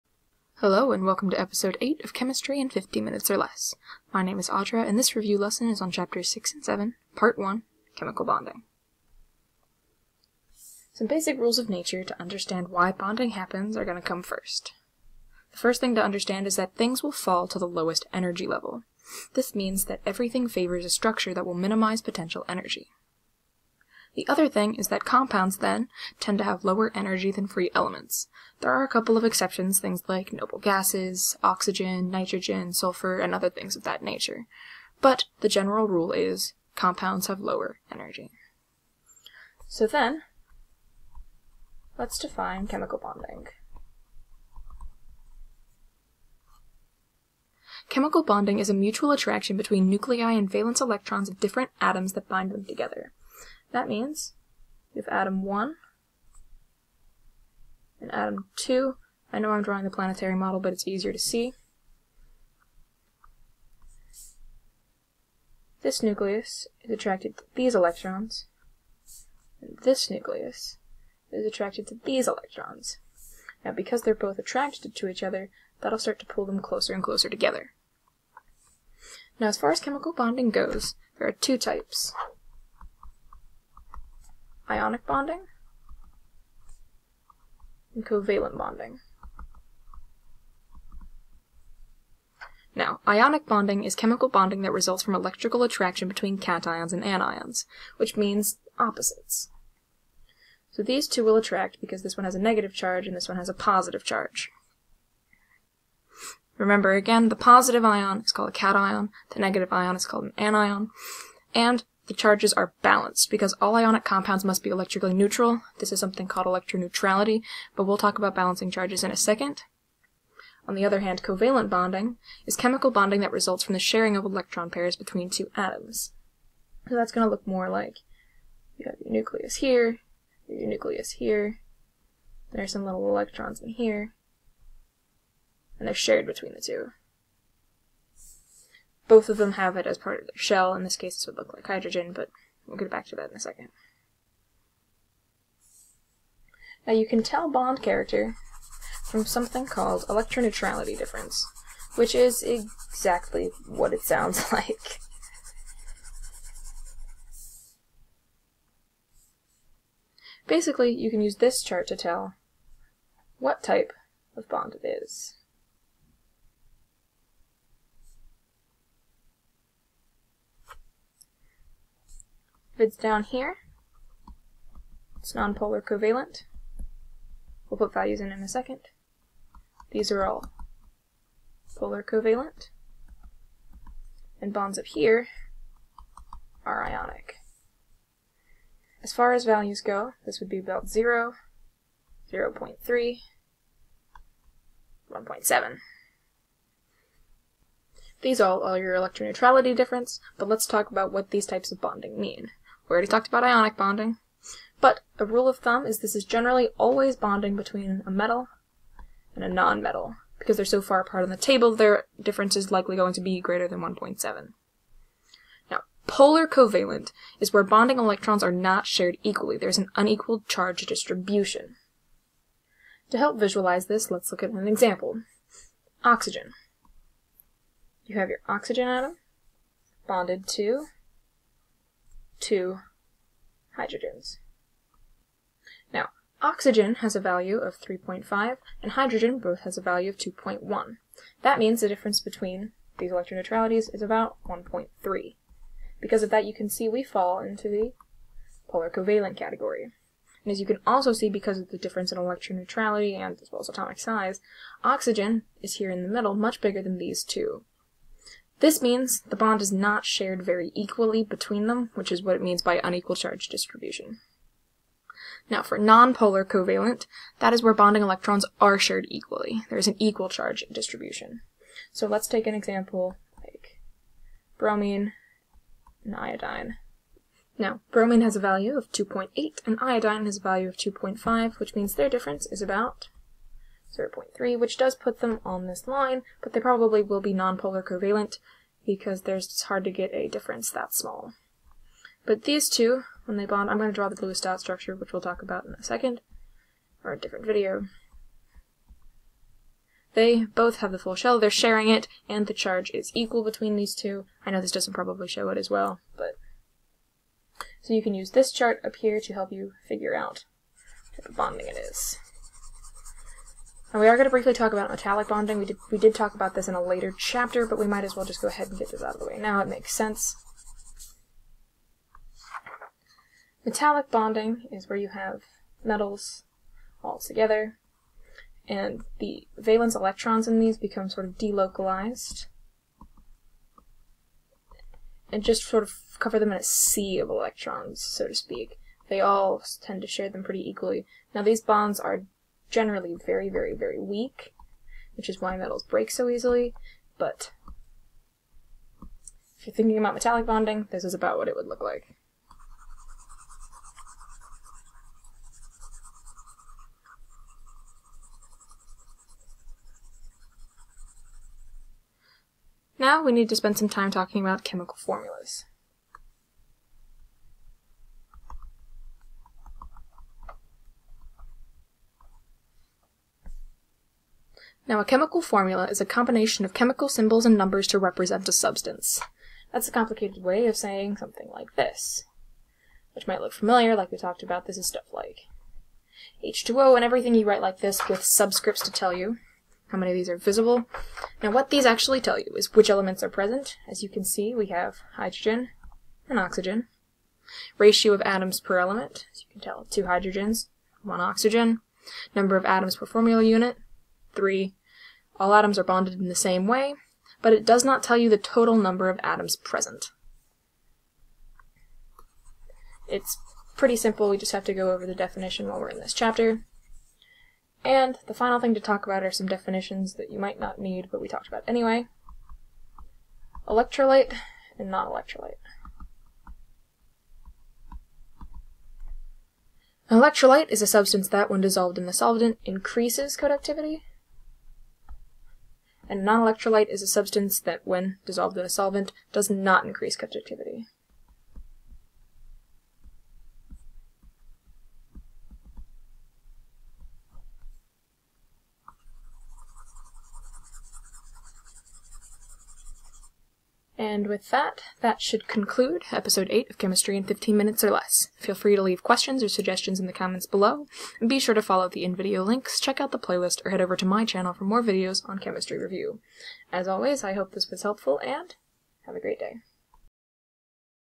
Chapters 6+7: Part I - Chemical Bonding (Chem in 15 minutes or less)
This is a quick review of some of the parts of my honors chemistry notes on chapters 6 and 7. There are some very important things in this video, including covalent and ionic bonding, determining bond character, and chemical formulas.
As always, hope this helps out.